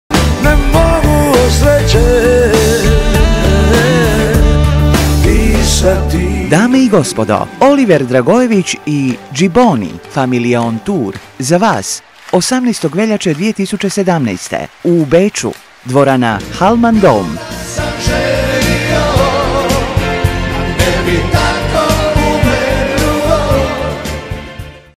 OLIVER & GIBONNI am 18.02.2017 live in Wien
Die kroatischen Sänger Oliver Dragojević und Zlatan Stipišić Gibonni treten am 18. Februar live im Wiener Hallmann Dome auf.